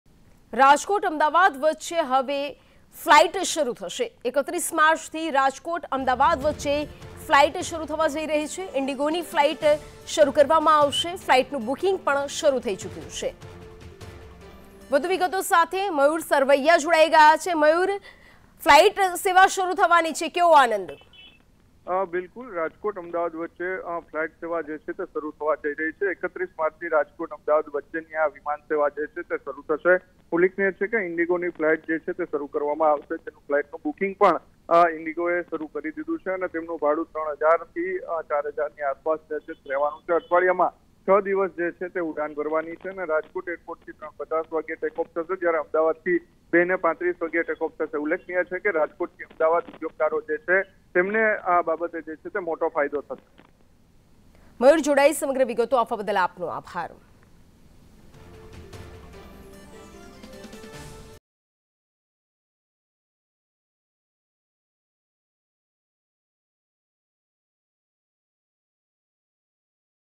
Breaking News | રાજકોટ અમદાવાદ વચ્ચે થશે ફલાઇટ શરૂ | Flight | Gujarat
રાજકોટ-અમદાવાદ વચ્ચે થશે ફલાઇટ શરૂ
31 માર્ચથી રાજકોટ-અમદાવાદ વચ્ચે ફલાઇટ શરૂ થશે
ઇન્ડિગોની ફલાઇટ કરવામાં આવશે શરૂ
ફલાઇટનું બુકિંગ પણ શરૂ થયું
અઠવાડિયામાં 6 દિવસ ઉડાન ભરશે
રાજકોટ એરપોર્ટ પરથી 3.50 વાગ્યે ટેક ઓફ થશે